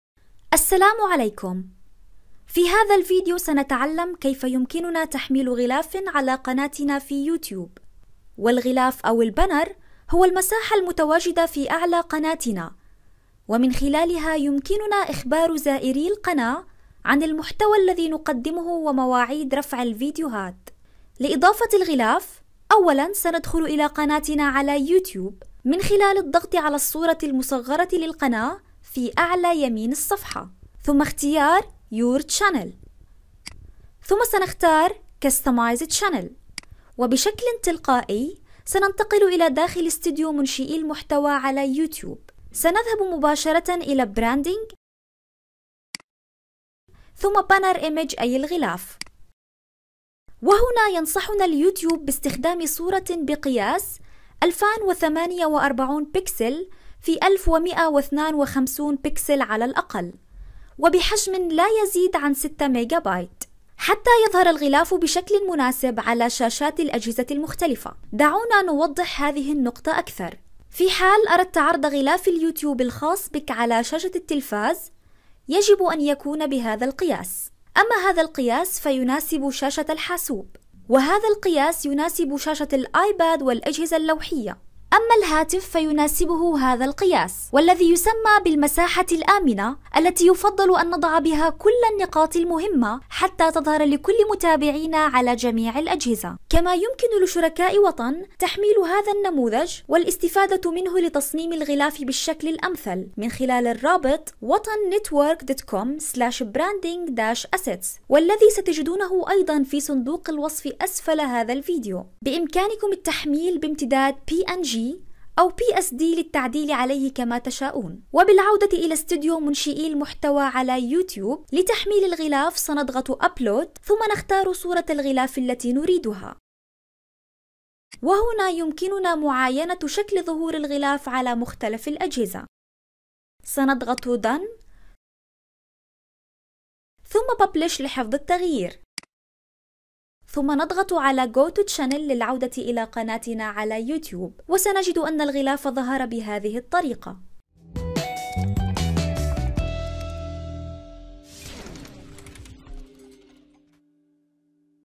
كيفية تحميل غلاف القناة على يوتيوب | WatanNetwork Help 2022
كيفية تحميل غلاف القناة على يوتيوب
How to add/edit YouTube banner

في هذا الفيديو سنتعلم كيف يمكننا تحميل او تعديل غلاف قناتنا على اليوتيوب من خلال ستوديو منشئي المحتوى ، غلاف اليوتيوب واحد من أهم العناصر التي تعكس مدى الاحترافية التي نظهرها تجاه زوار القناة الجدد و الذي يساعدهم على اتخاذ قرار الاشتراك في القناة .

عندما تختار تصميما لغلاف قناتك على اليوتيوب يفضل أن يحتوي هذا التصميم على اسم القناة ،المحتوى الذي تنوي تقديمه في قناتك ، مواعيد رفع الفيديوهات ، اشارة إلى مواقع التواصل الإجتماعي التي تتواجد عليها ، كل هذه الأمور من شأنها مساعدة زائر قناتك على الضغط على زر الاشتراك و الإنضمام إلى جمهورك .

لتحميل نموذج تصميم غلاف القناة الاحترافي من موقع وطن

فريق شبكة وطن ، معاً نحقق المستحيل.
Watan Network Help